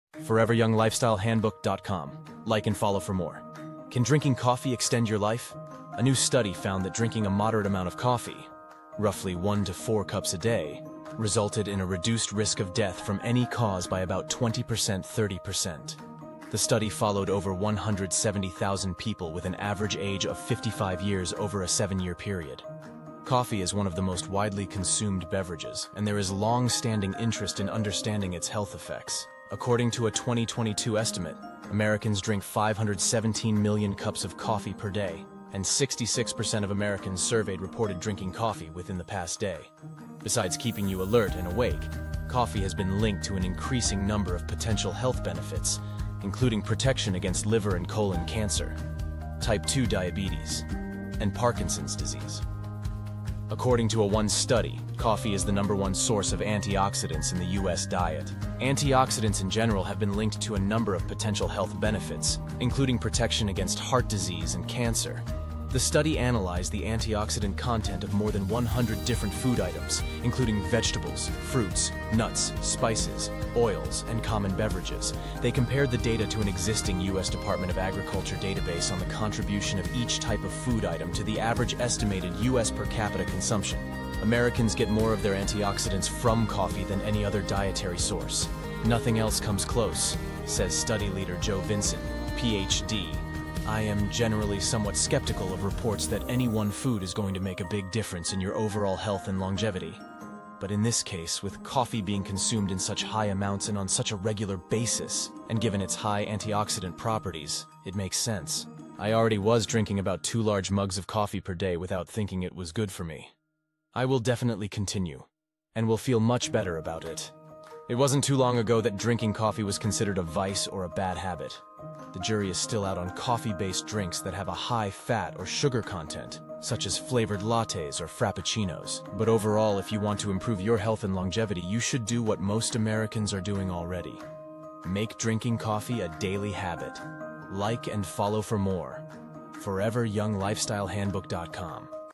Can Drinking Coffee Extend Your Life?
A new study found that drinking a moderate amount of coffee—roughly 1 to 4 cups a day—resulted in a reduced risk of death from any cause by about 20%-30%. The study followed over 170,000 people with an average age of 55 years over a 7-year period.

Coffee is one of the most widely consumed beverages, and there is longstanding interest in understanding its health effects. According to a 2022 estimate, Americans drink 517 million cups of coffee per day, and 66% of Americans surveyed reported drinking coffee within the past day. Besides keeping you alert and awake, coffee has been linked to an increasing number of potential health benefits, including protection against liver and colon cancer, type 2 diabetes, and Parkinson’s disease.

According to a one study, coffee is the number one source of antioxidants in the U.S. diet. Antioxidants in general have been linked to a number of potential health benefits, including protection against heart disease and cancer. The study analyzed the antioxidant content of more than 100 different food items, including vegetables, fruits, nuts, spices, oils and common beverages. They compared the data to an existing U.S. Department of Agriculture database on the contribution of each type of food item to the average estimated U.S. per capita consumption. “Americans get more of their antioxidants from coffee than any other dietary source. Nothing else comes close,” says study leader Joe Vinson, Ph.D.

I am generally somewhat sceptical of reports that any one food is going to make a big difference in your overall health and longevity. But, in this case, with coffee being consumed in such high amounts and on such a regular basis—and given its high antioxidant properties—it makes sense. I already was drinking about 2 large mugs of coffee per day, without thinking it was good for me. I will definitely continue—and will feel much better about it. It wasn’t too long ago that drinking coffee was considered a vice or a bad habit. The jury is still out on coffee-based drinks that have a high fat or sugar content such as flavored lattes or frappuccinos. But overall, if you want to improve your health and longevity, you should do what most Americans are doing already: make drinking coffee a daily habit.

Forever Young Lifestyle Handbook dotcom

K5Fi1BuU: CJLH71ULNMTBE2WL
K5Fi1BuU: Q81ARN9M2CLGVOMM